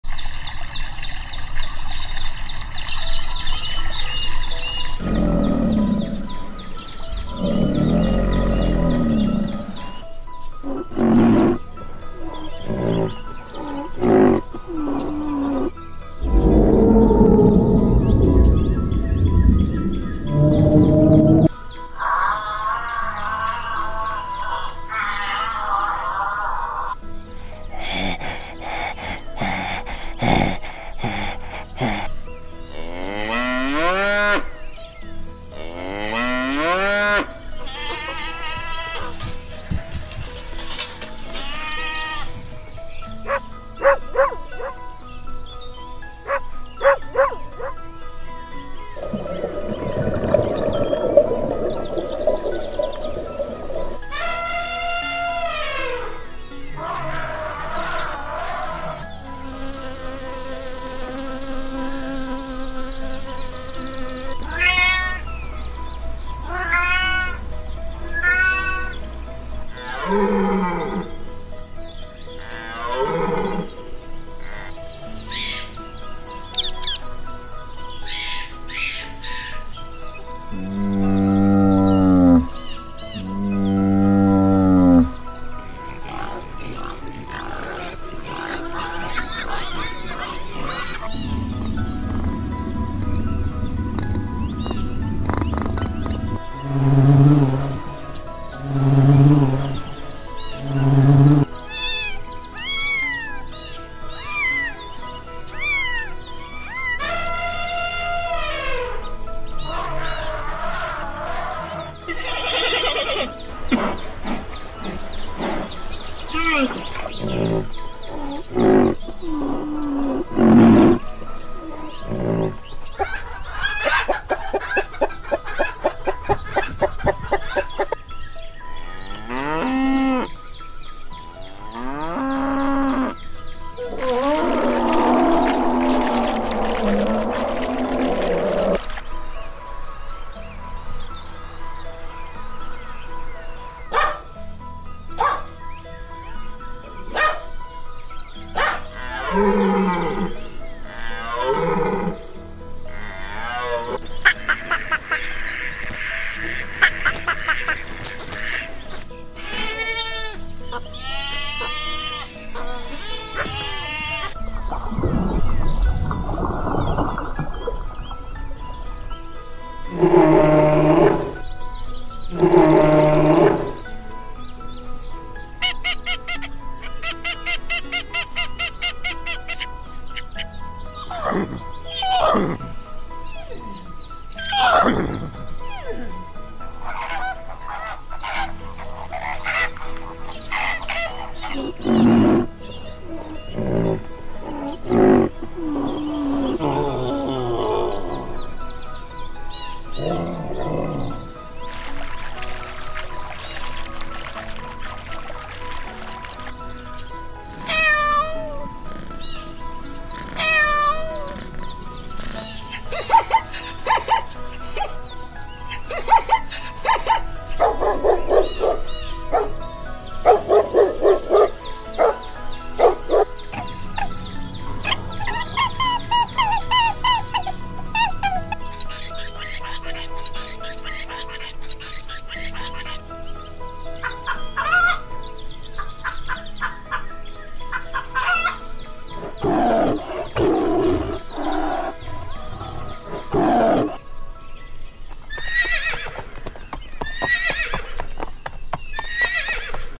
Wild animals make a wide range of noises: crocodiles, rhinos, whales, girrafes, hyenas,…
We'll be learning about some of the world’s most iconic and well-known animals. How animals look like, their habits, and the range of noises they make. Birds chirping and relaxing music background is a wonderful way to provide a calming and comforting experience while watching this video and is ideal for sleep, study, meditation, and stress relief.
This is what spurs us on to make channel improvements. Thank you for your support!